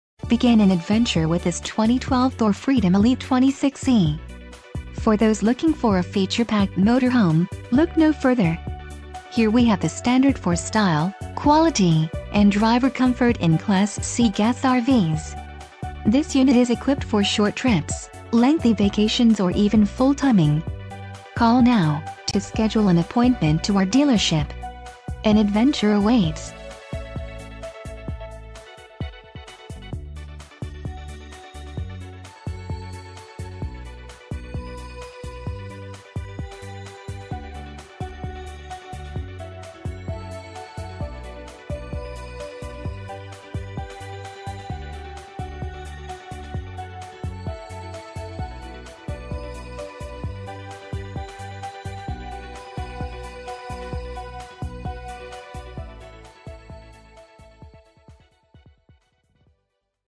2012 Thor Freedom Elite - Camping World of Myrtle Beach - M
Camping World of Myrtle Beach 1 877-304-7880

 Booth Dinette, Bunk over Cab, Double door refrigerator, Ducted AC, Ford Chassis, Grab Handle, Microwave, Mirrored Wardrobe, Power Windows  and  Locks, Rear Corner Bath, Rear Corner Bunk,